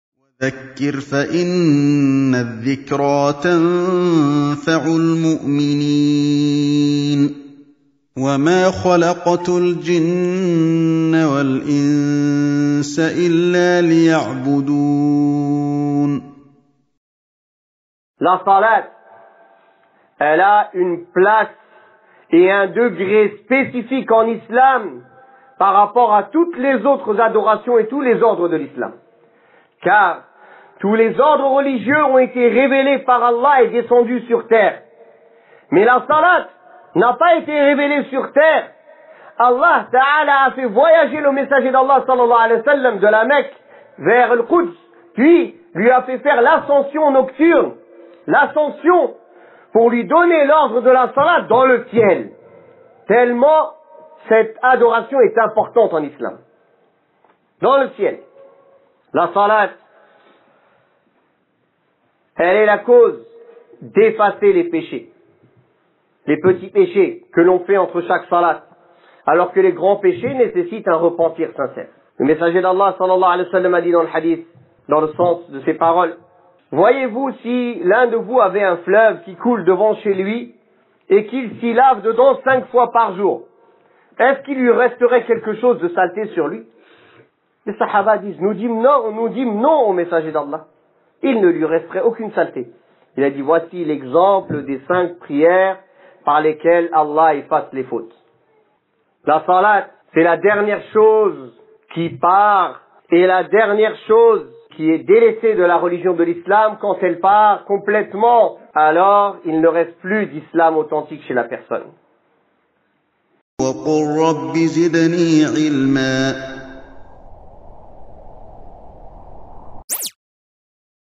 🕌 L'importance de la salat (Abou Laïth 'Othmân Al Armany) - Dourous-Sounnah.com
Extrait #272 : L'importance de la salat

Khotba/sermon du vendredi: La prière.
27 Novembre 2015